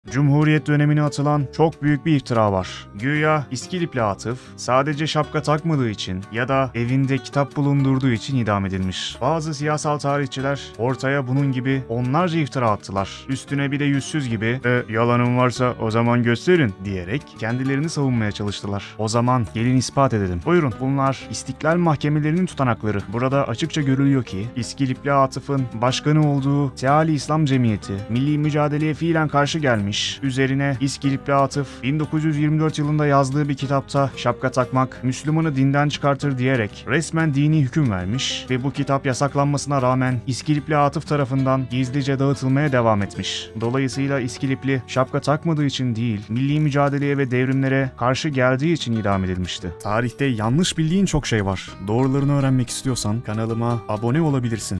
Şapka Takmayanlar Asıldı!
Cumhuriyet dönemine atılan çok büyük bir iftira var!

Güya İskilipli atıf, sadece şapka takmadığı için ya da evinde kitap bulundurduğu için idam edilmiş. Bazı siyasal tarihçiler, ortaya bunu gibi onlarca iftira attılar. Üstüne bir de yüzsüz gibi, "E yalanım varsa o zaman gösterin!" diyerek kendilerini savunmaya çalıştılar.
O zaman gelin ispat edelim. Buyrun, bunlar İstiklal Mahkemelerinin tutanakları. Burada açıkça görülüyor ki, İskilipli Atıf'ın başkanı olduğu Teal-i İslam cemiyeti Milli Mücadeleye fiilen karşı gelmiş, üzerine İskilipli Atıf'ın 1924 yılında yazdığı bir kitapta: "Şapka takmak Müslüman'ı dinden çıkartır" denerek resmen dini hüküm verilmiş. Üstelik bu kitap yasaklanmasına rağmen İskilipli Atıf tarafından gizlice dağıtılmaya devam etmişti.

Dolayısıyla İskilipli, şapka takmadığı için değil, Milli Mücadeleye ve devrimlere karşı olduğu için idam edilmişti.

Kaynakça: İstiklal Mahkemesi Tutanakları, Erol Şadi Erdinç de Murat Bardakçı'nın bir programında bu durumdan bahsetmiş.